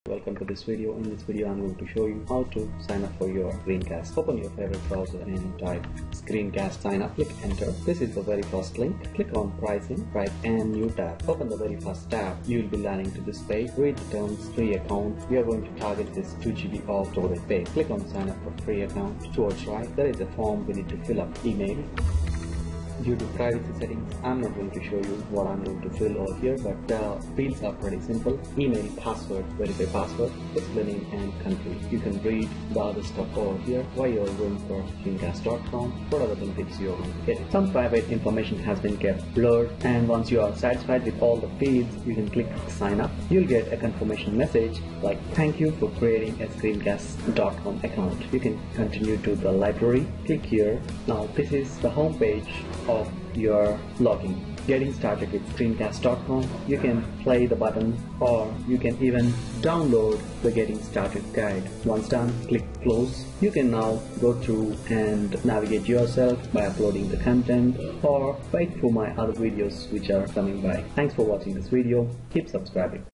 Register to ScreenCast.com for Free
Registering to ScreenCast.com as Free Account.

This video is targetted to novice Computer viewers. This is pretty simple and straight forward in demonstration.

Watch this video and get a free ScreenCast account. This account helps you to upload all your favourite Videos and photo content in single place.

Website is offering 2GB of free space montly. If you feel this is small you can buy Pro account which expands to 25GB of space.

The more people are getting beneficial.. I will keep producing videos.

Thanks
Kiran Ginni